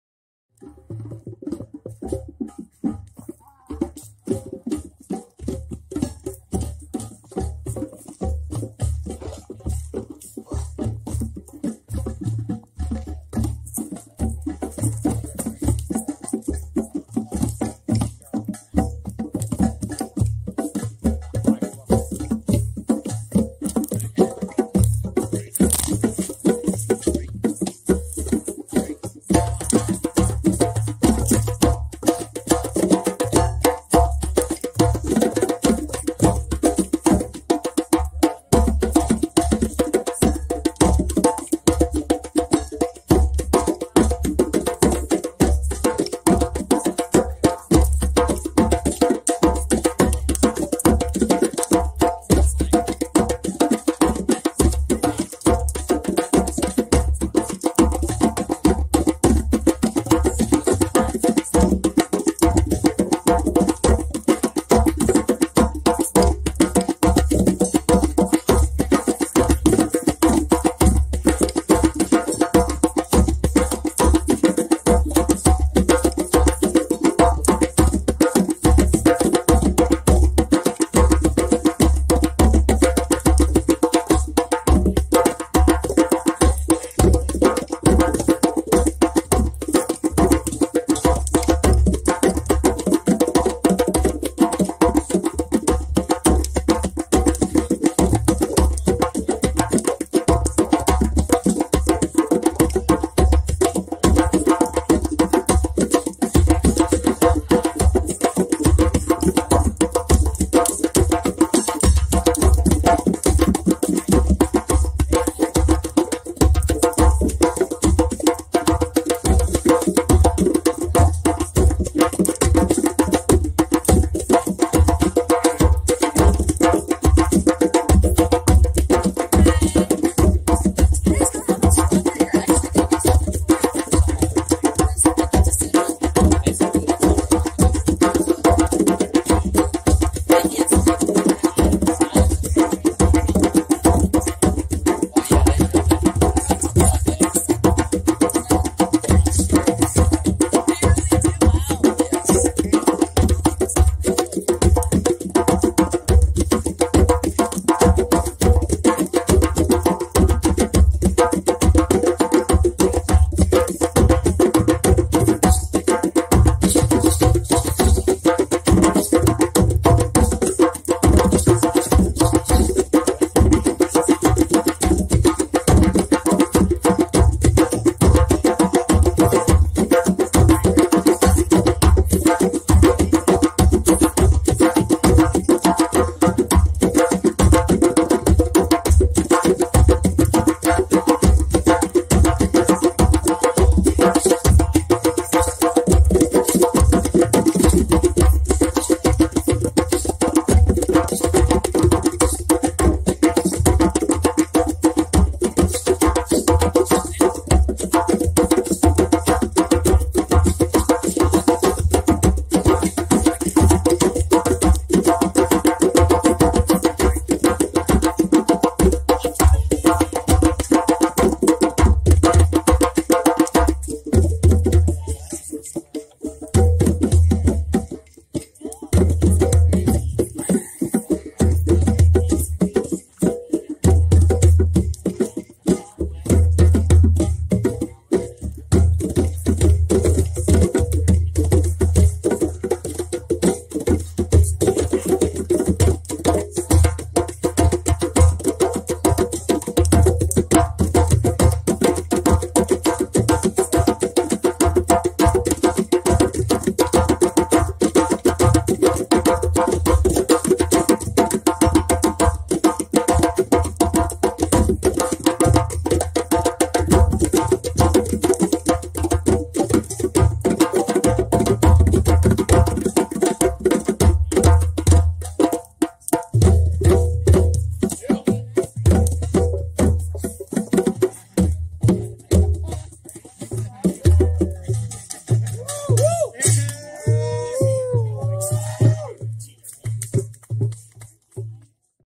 Hi Do You Guys Know How Much Longer You're Actually Going To Be Drumming For?
from Eeyore's Unbirthday Afterparty's Afterparty's Pink Moon full moon drum circle at Texas Music Ranch, Austin Texas